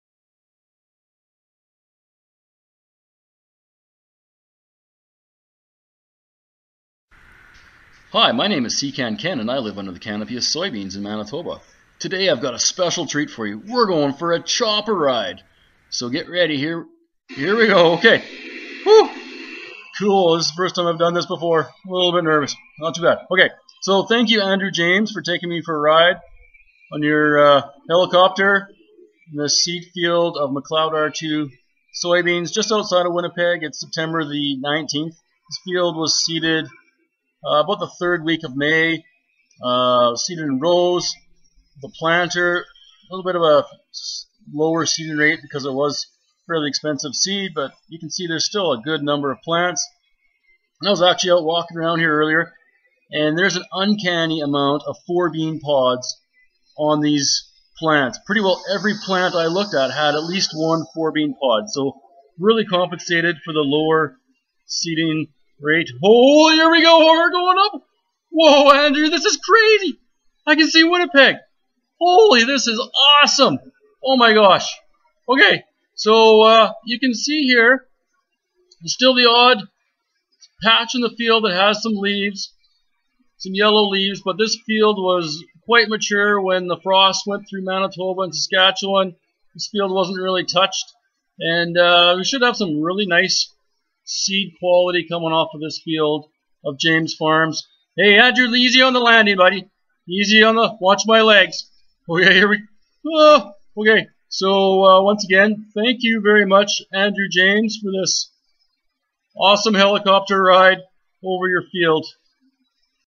SeCan Ken Takes flight!
Sept 19 SeCanKen goes for a helicopter ride over a field of McLeod R2 Soybeans